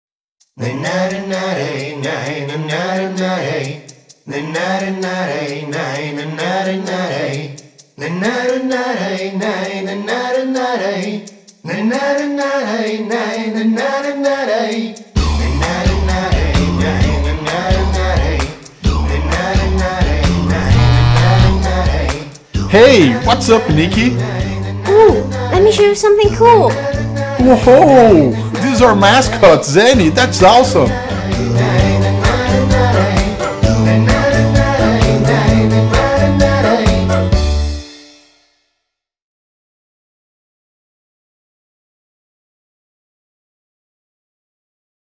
ZeniMoji, a fun way to communicate - ZenFone 5 Series | ASUS
ZeniMoji is a fun new ZenFone 5 feature — exclusive to ASUS — that lets users animate cute avatars using their own voice, facial expressions and head movements. Uniquely, ZeniMoji avatars can be used in video chats or live-streaming, as well as in text chats, to make them more entertaining!